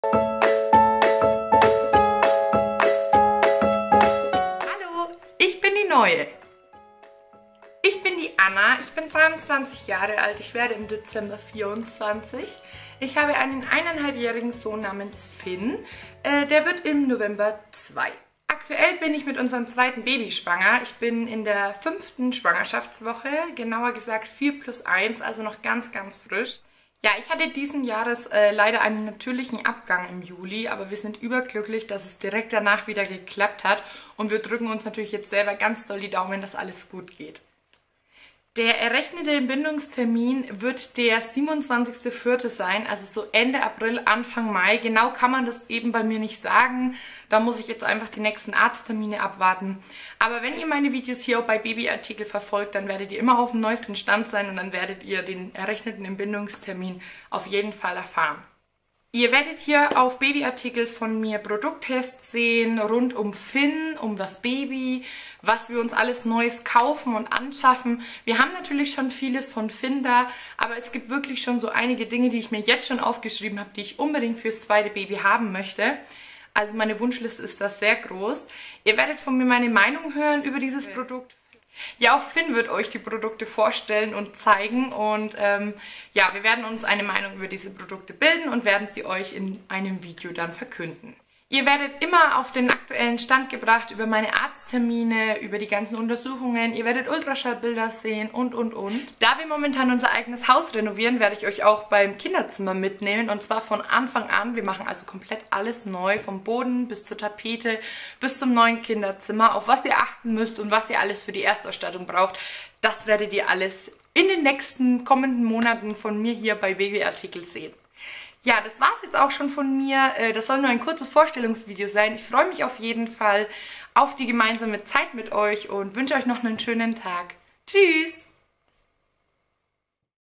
Die NEUE stellt sich vor | Babyartikel.de
Hallo ich bin Anna (23),

Mama von Fynn (1,5) und zur Zeit mit Baby Nr. 2 schwanger. In diesen Video stelle ich mich euch vor und erzähle, über was ich in der kommenden Zeit berichten werde!